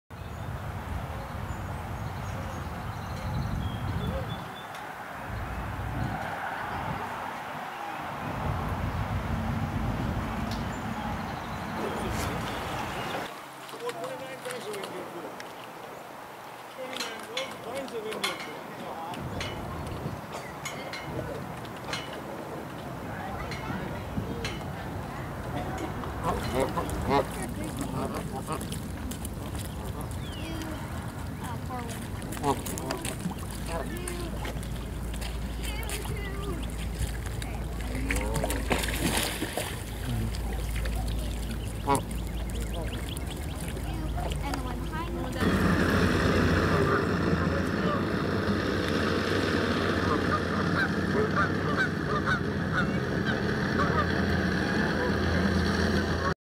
Somewhere on the Chattahoochee River - Sony HDR-TD 20 Test Video
Somewhere on the Chattahoochee River. Sony HDR-TD 20 Test Video. February 9, 2014.